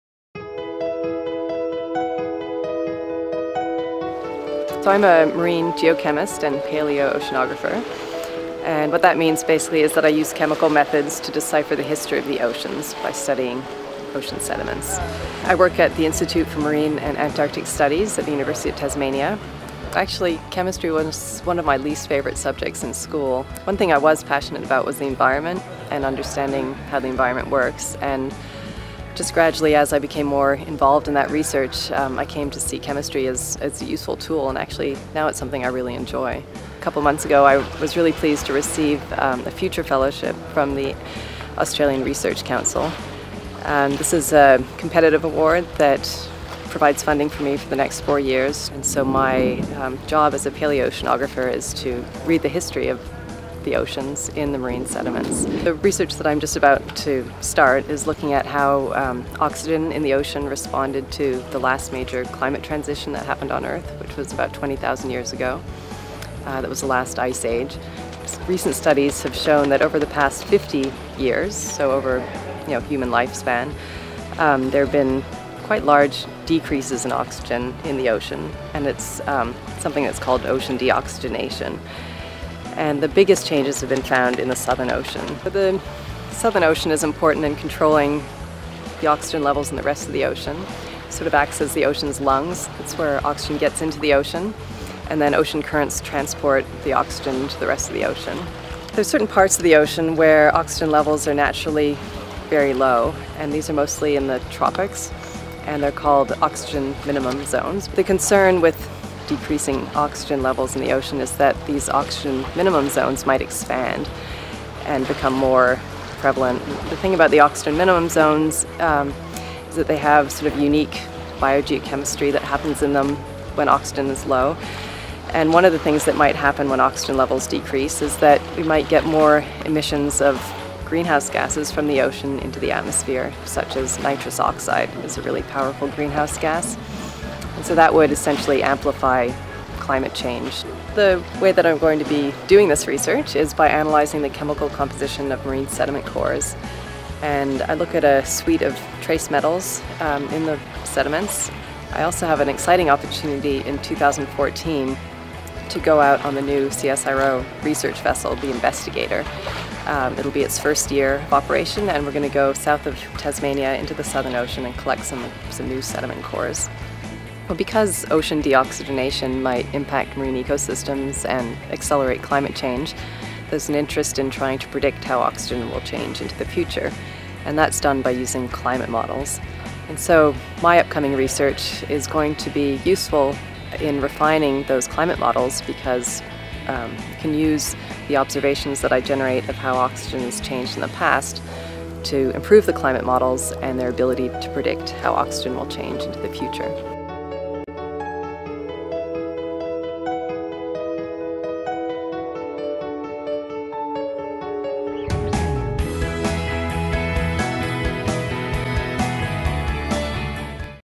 IMAS - Zanna Chase's Ocean Oxygen
Zanna Chase talks about her research on ocean oxygen levels as an ARC Future Fellow at the Institute for Marine and Antarctic Studies (IMAS) at UTAS. The Bookend Trust is pleased to partner with IMAS to bring these research profiles to the public.